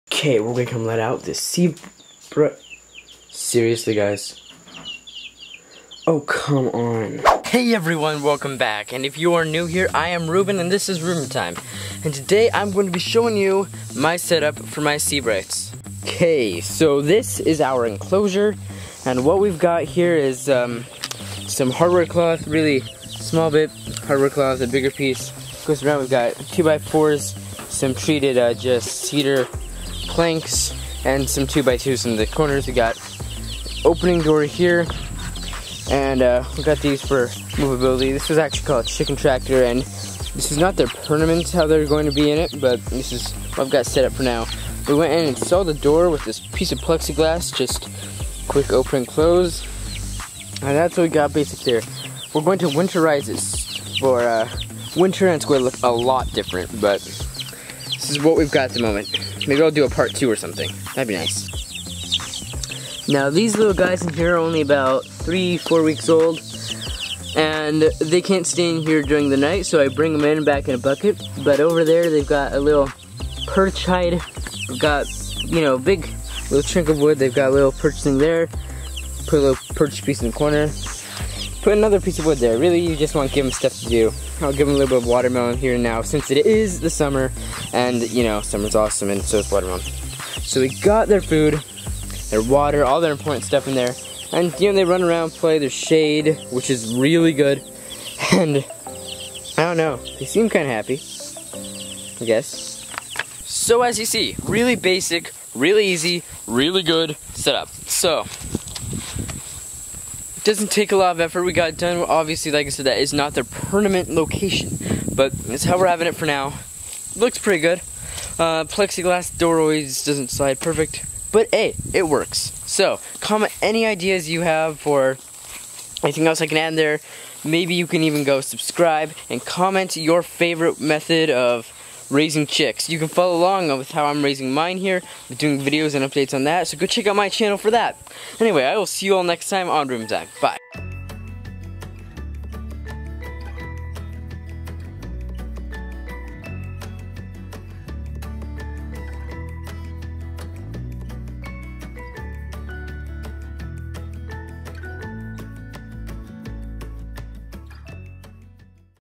Chicken enclosure setup!
Today I will be showing you the setup for my tiny chicken enclosure! Comment any ideas you have to make it better!

Uploads on Tuesdays and Thursdays at 4:00 P.M. Weekly